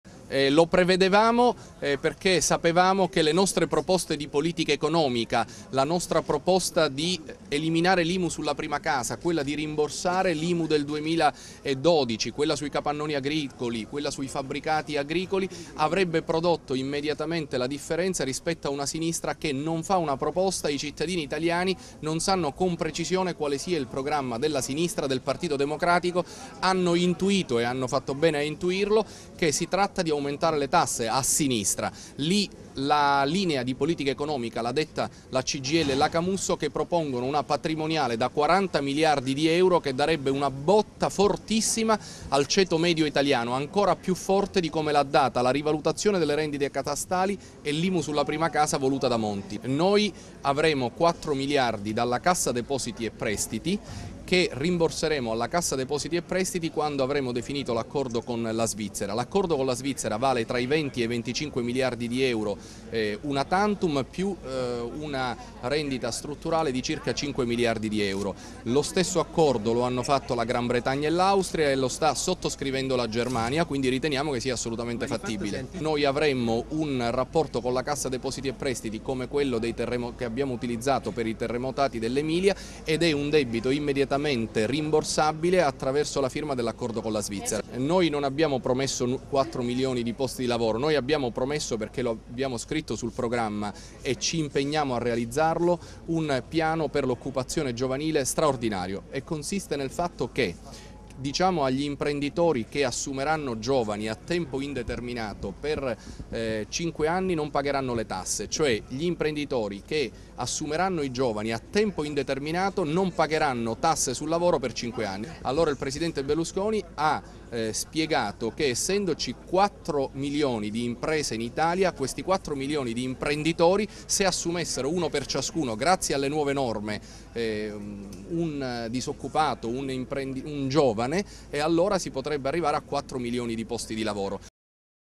Alfano in Abruzzo per il PDL
Questa mattina a Pescara, all'auditorium Petruzzi, visita del segretario nazionale del PDL Angelino Alfano. Presenti tutti gli esponenti politici pdiellini abruzzesi e tanti militanti